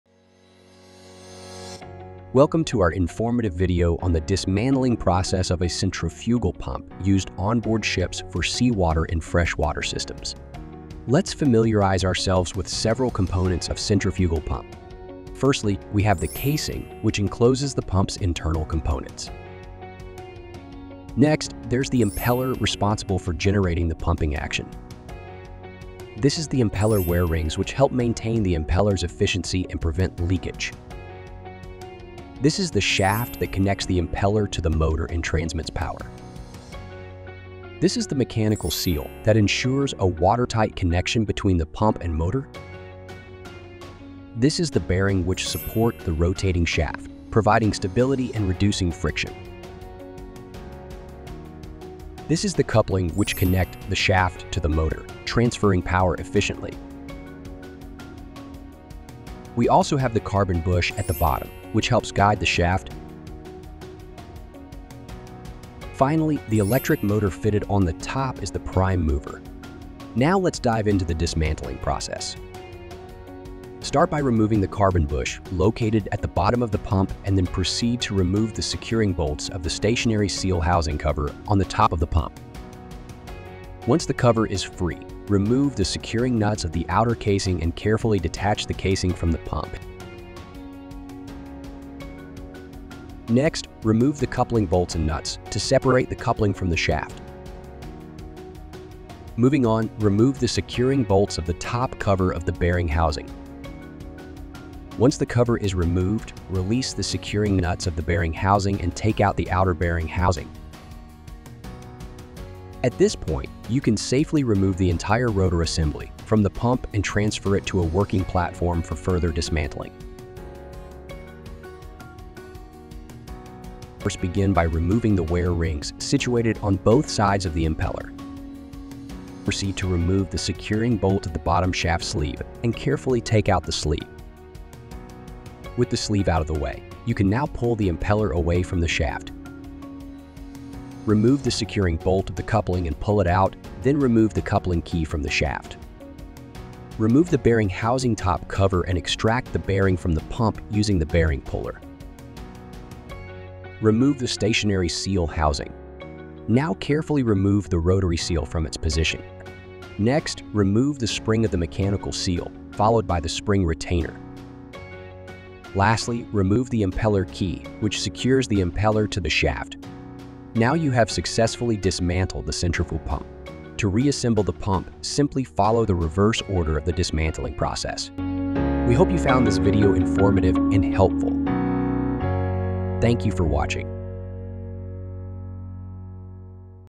Centrifugal Pump | 3D Animated Explanation | HIMT
Explore the fascinating mechanics of centrifugal pumps in our detailed 3D animated video! Discover the essential components such as the impeller, casing, shaft, and seals, and learn their specific functions. Follow our step-by-step guide to dismantling the pump, with clear instructions that make understanding maintenance procedures easy. Perfect for students, engineers, and enthusiasts, this video is your go-to resource for deepening your knowledge of fluid dynamics and pump mechanics.

Faculty Mentor: S. Paranthaman